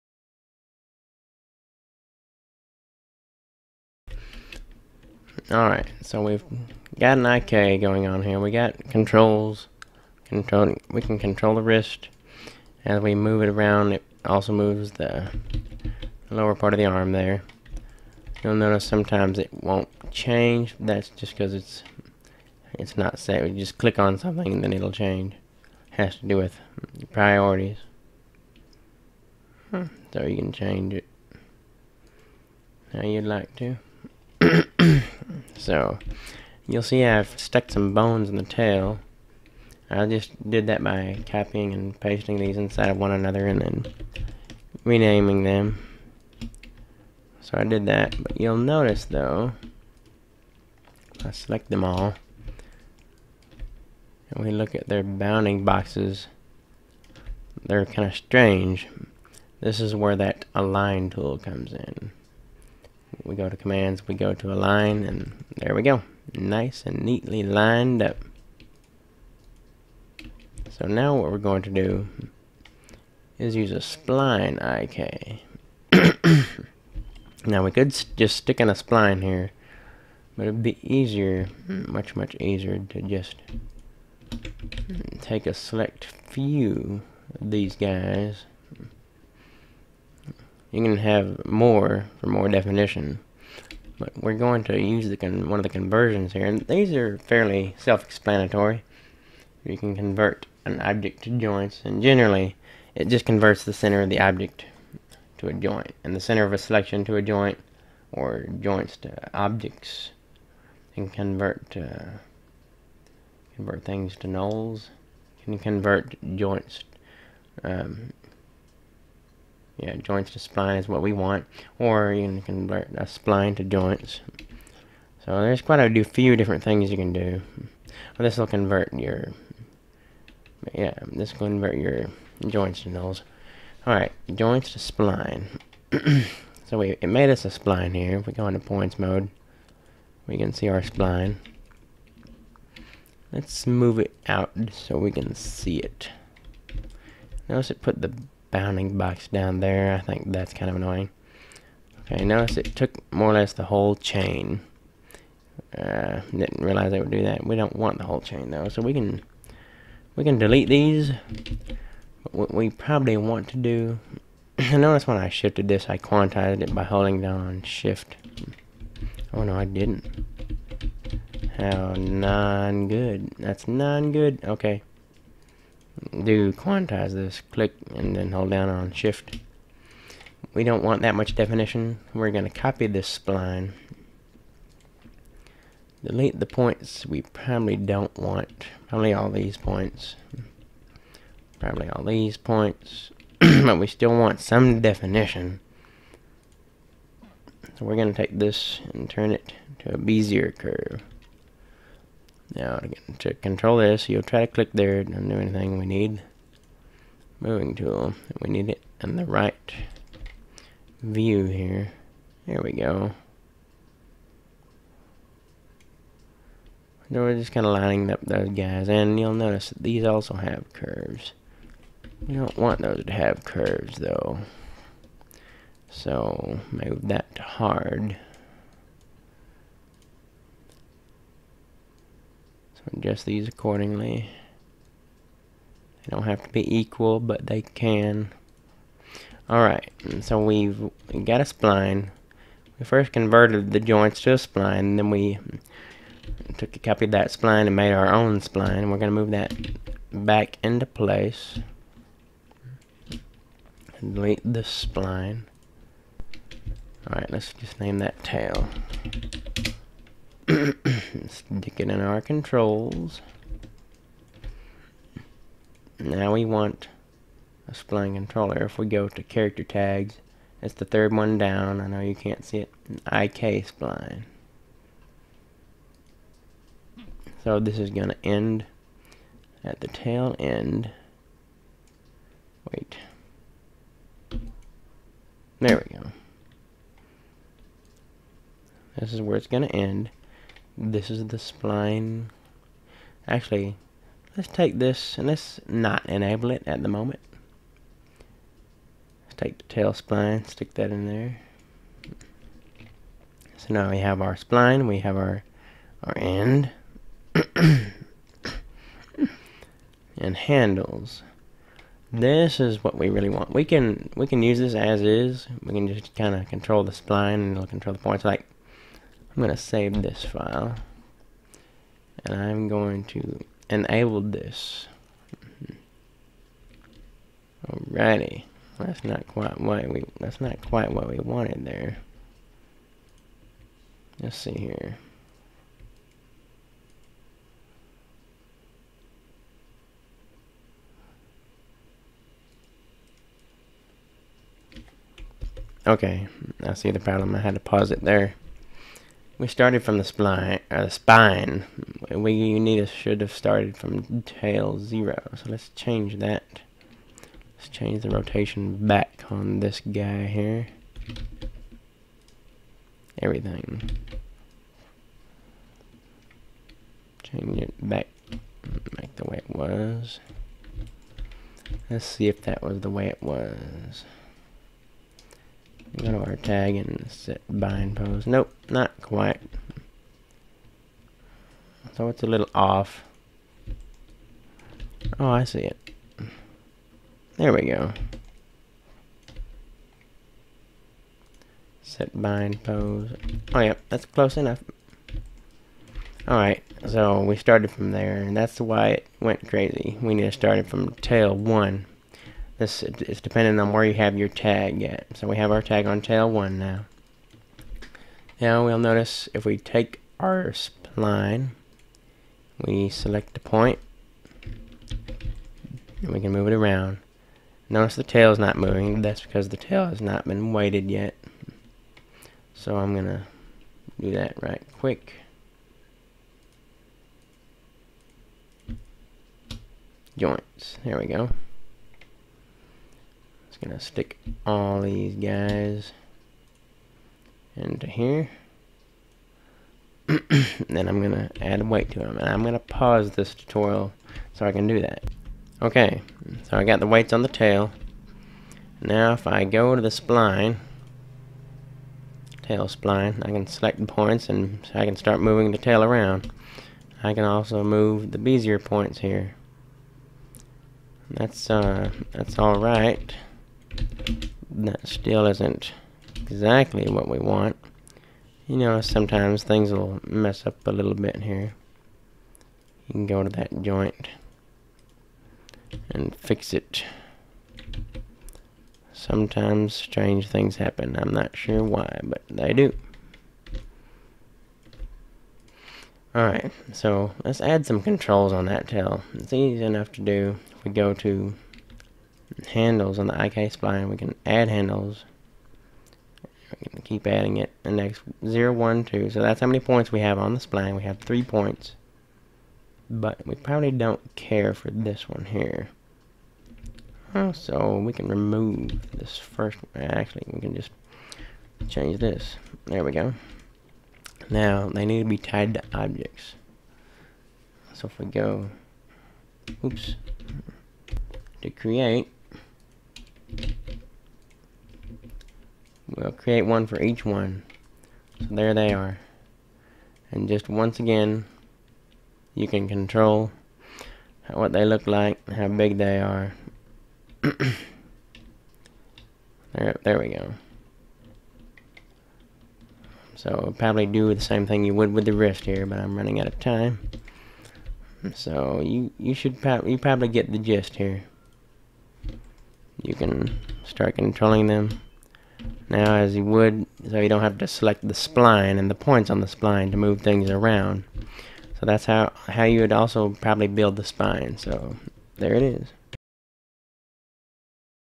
Cinema 4D Rigging 05 - Creating an IK-Spline with Controls
This tutorial describes how to setup an IK-Spline with controls to use for animation.